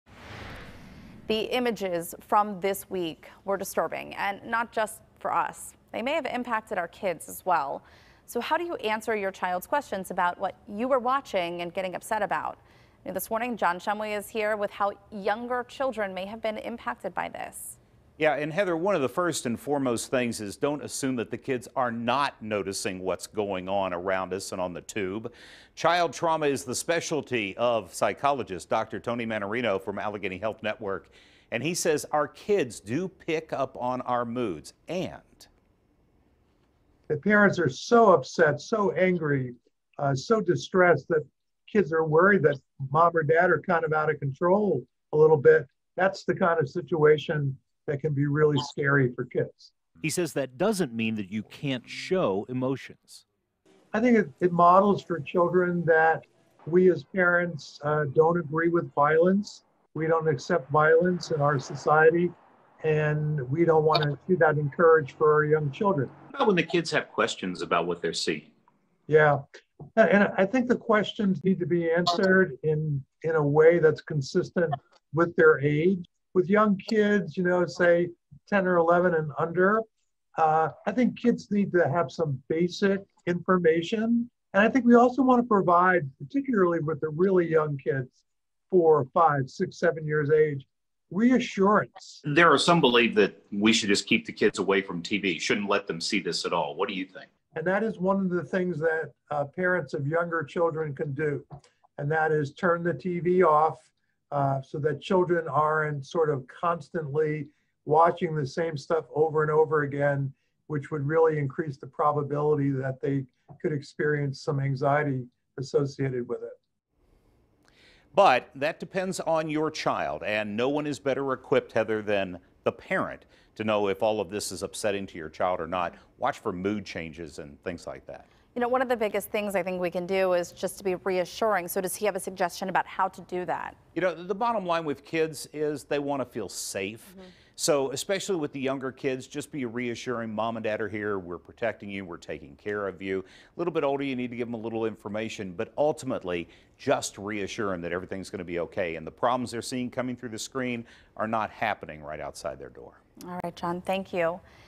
Keeping Children Informed And Calm During Tumultuous Times
While many of us were shocked and disturbed at what we saw at the U.S. Capitol on Wednesday, so were our kids. So KDKA's John Shumway reached out to Dr. Tony Mannarino a child psychologist at Allegheny Health Network to talk about how to keep your children informed but also calm.